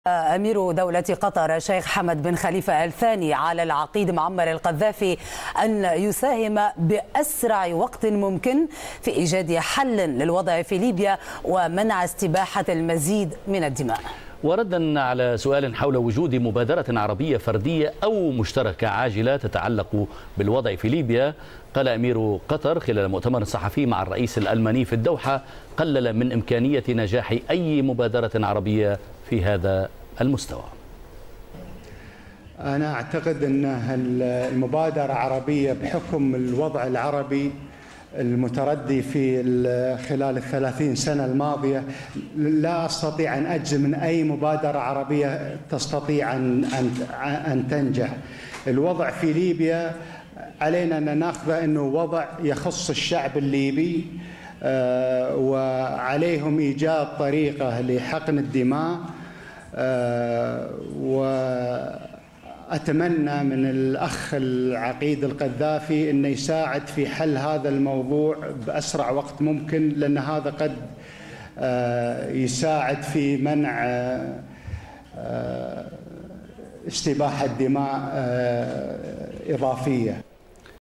Sheikh Al Thani.flv
الامير حمد ال ثاني يتمنى على القذافي ايجاد طريقة لحقن دماء الليبين